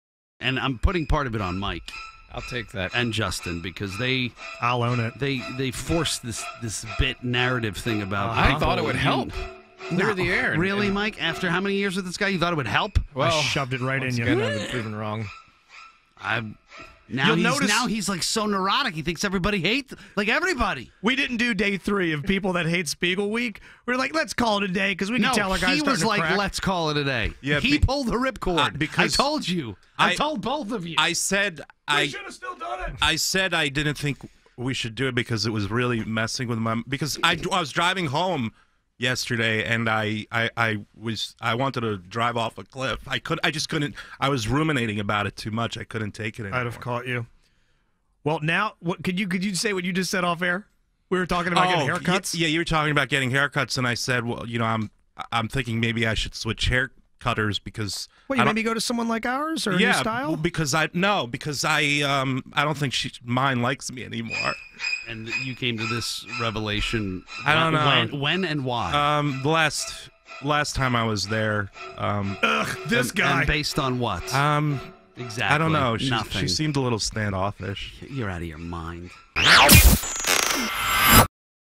People Who Hate Spiegel Week: Why No Day 3?
Our own Josh Spiegel has a list of people that he thinks don’t like him in the office. This week, we had hoped to confront these people and figure out if any of these people have a problem with Josh. But today, we decided to bring the bit to an end.